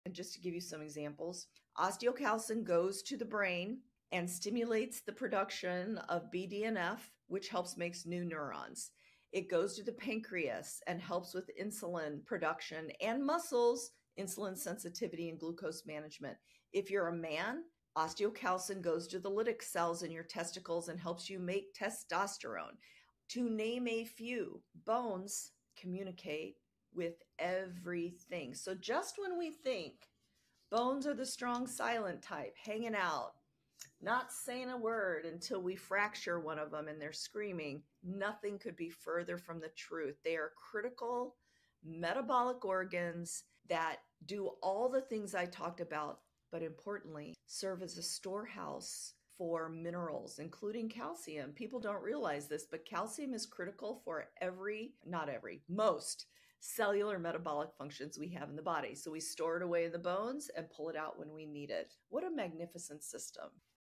Bones: The Unsung Heroes of Your Body's Communication Network!
Bones aren't just silent supporters; they're active communicators! Osteocalcin stimulates BDNF, aiding neuron production. Bones boost insulin, manage glucose, and even help testosterone production. They're mineral storehouses too, critical for cellular functions.